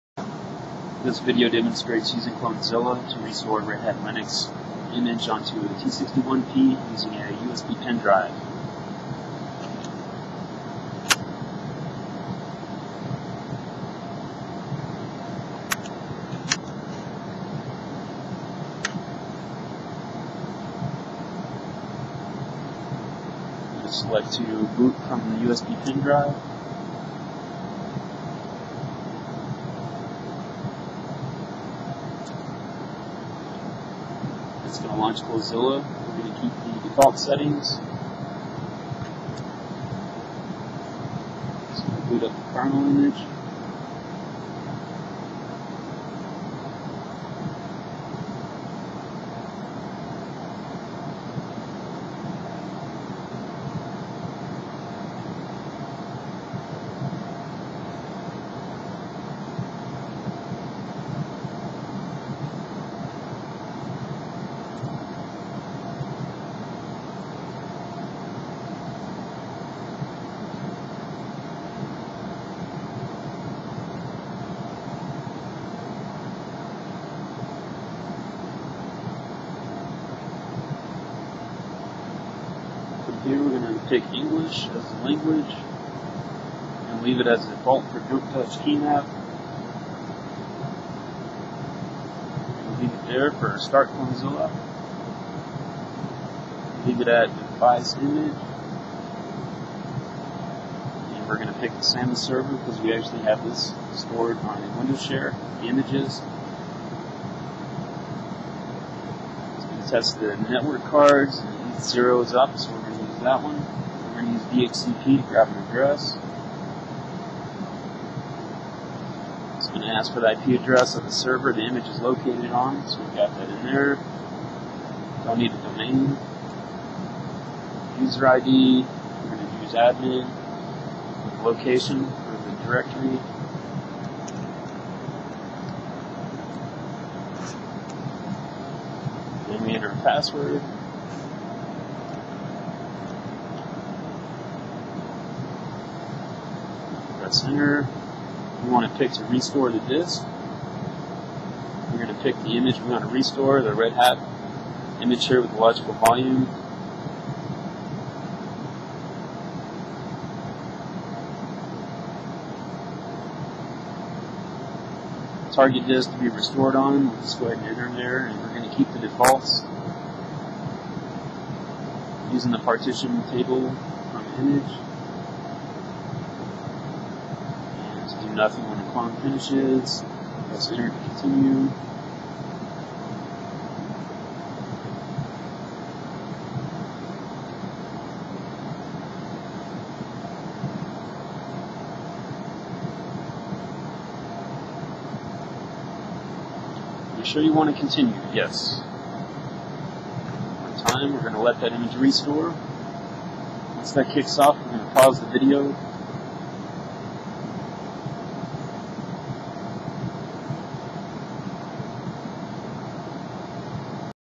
Red Hat 5 Linux Image Restore Using Clonezilla Part 1
Restoring a Red Hat 5 Linux image on a T61P laptop using Clonezilla on a USB Pen Drive
Part 1